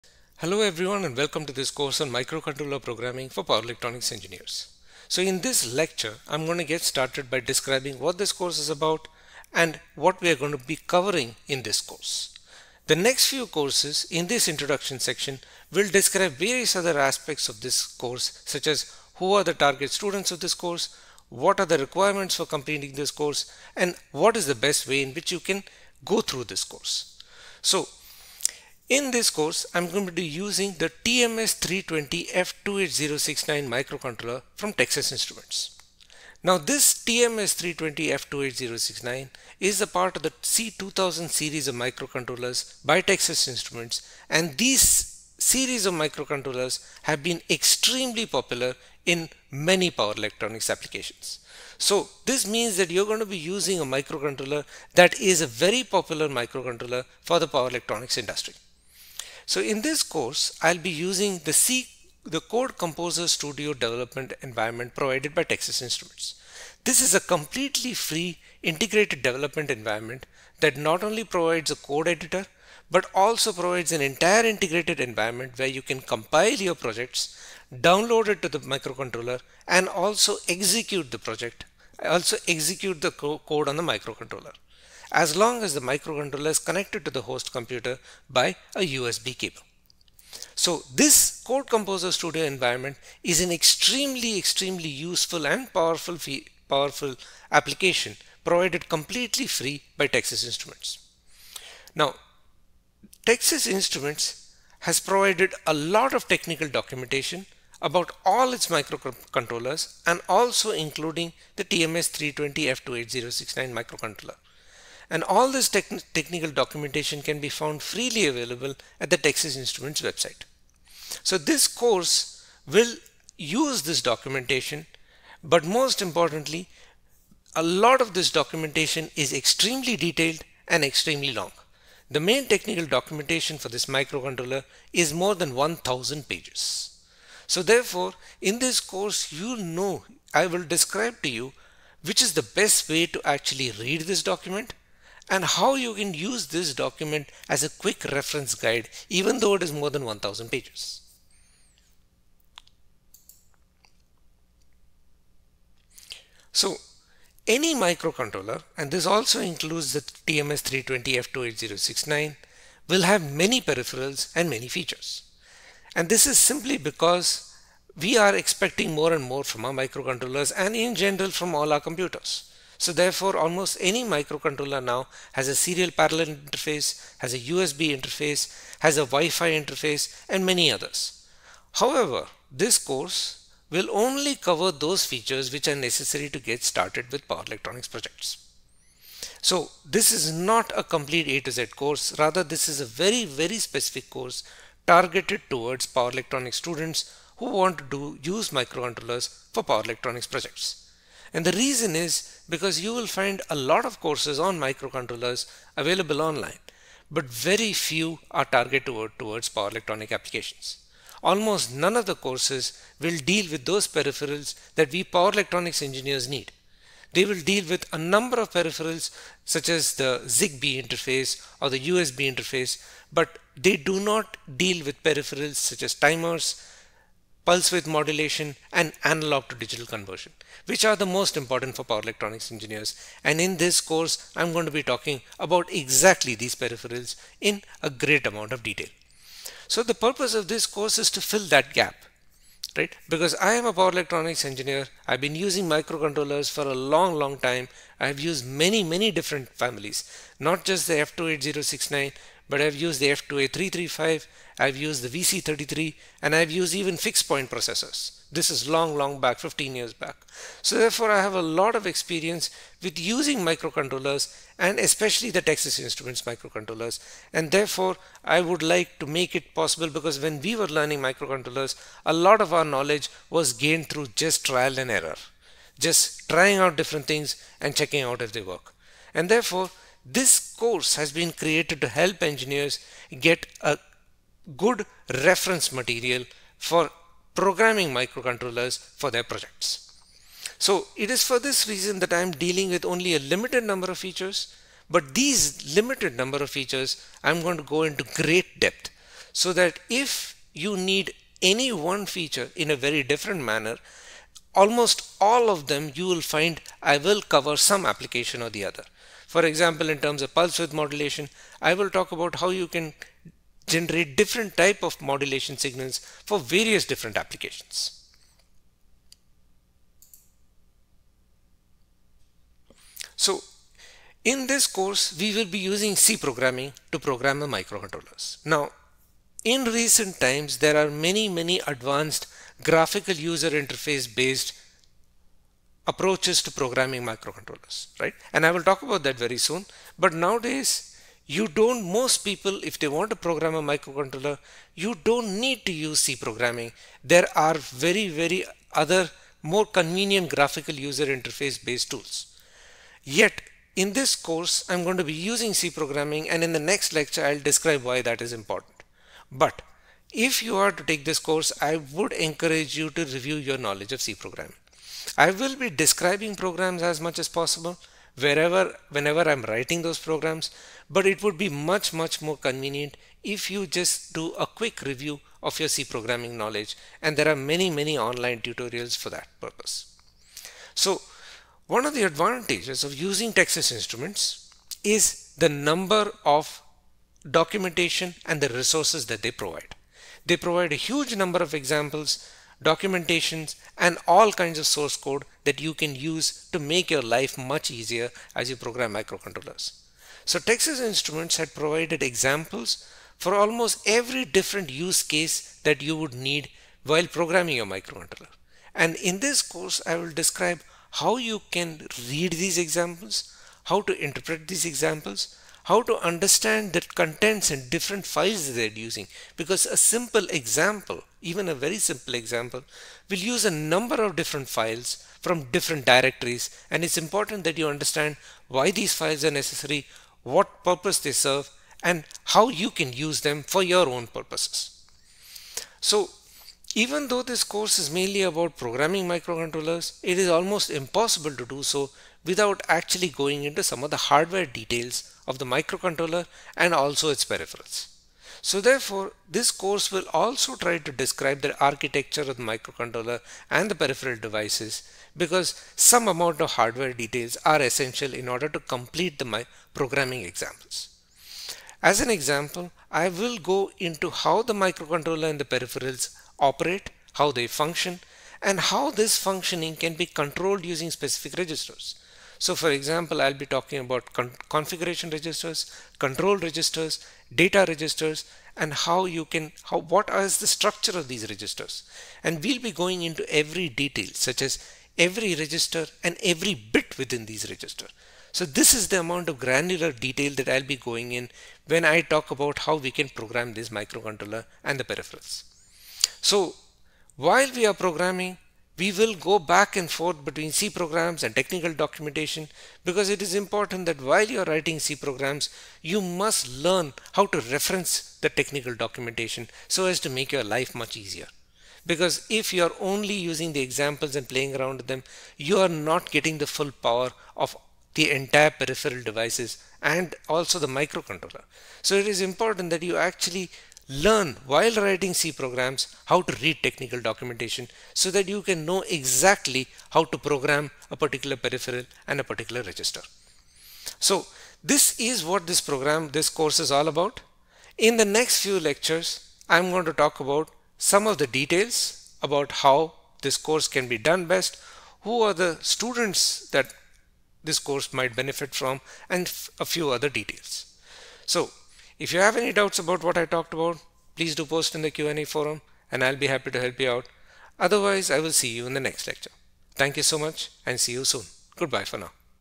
Microcontroller Programming for Power Electronics Engineer
This is the very first welcome lecture where I provide a basic overview of the course.